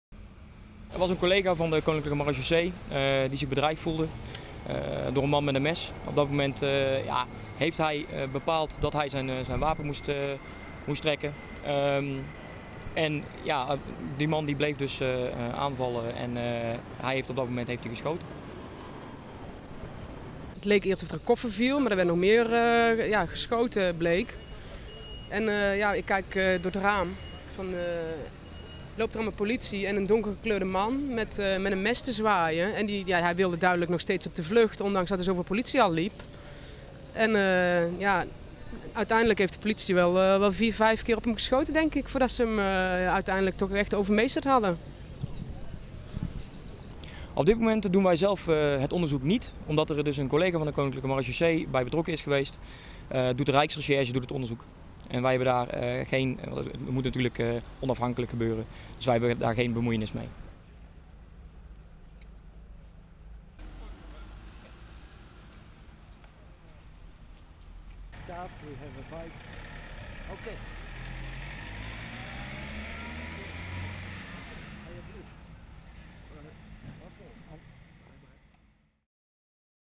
Marechaussee schiet man neer op Eindhoven Airport
Voor de terminal van Eindhoven Airport is vrijdagmorgen een man neergeschoten door de koninklijke marechaussee. De man liep met een mes te zwaaien maar weigerde dat na herhaaldelijke waarschuwingen neer te leggen. Toen hij een marechaussee ermee bedreigde was de maat voor de agenten vol en werd de man in zijn been geschoten. De 30-jarige Pool is met onbekend letsel overgebracht naar het ziekenhuis. De rijksrecherche onderzoekt het incident, het technisch onderzoek word verricht door specialisten van de politie Brabant Zuid-Oost.